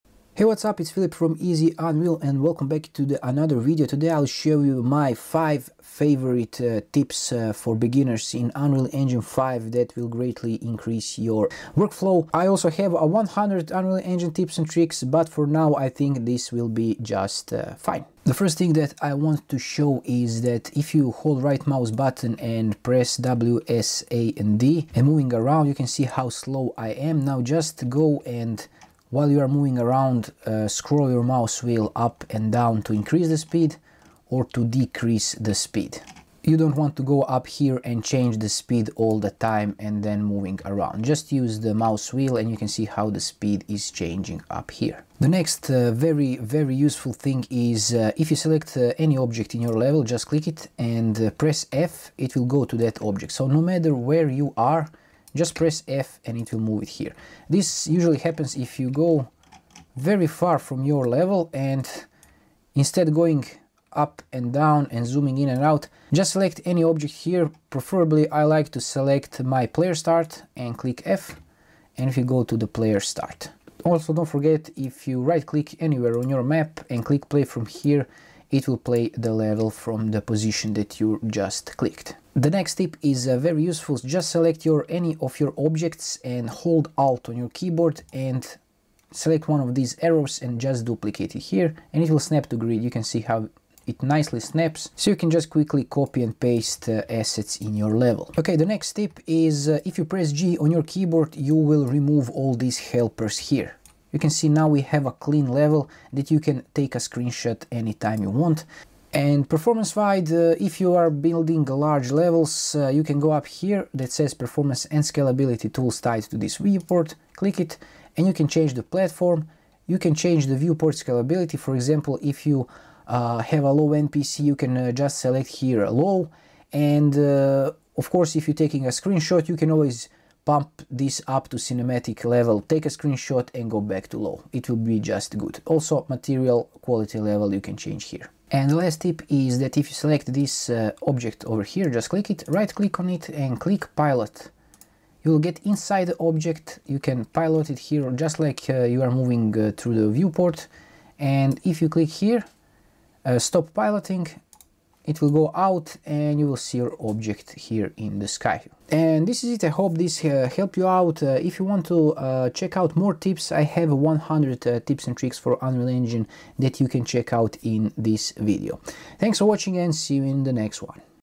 5 Must-Know Unreal Engine 5 Tips for Beginners (Boost Your Workflow Fast)
In this video, I’m sharing my five favorite Unreal Engine 5 beginner tips — simple but powerful tricks that instantly improve your workflow.

You’ll learn how to:
• Move faster in the viewport
• Snap to objects instantly
• Duplicate assets the right way
• Hide helpers with one key
• Boost performance in large levels
• Pilot objects for quick previews

These are the exact tips I still use every day, and they’ll save you time, frustration, and tons of clicking. If you want even more, I also have 100 Unreal Engine tips & tricks you can check out next!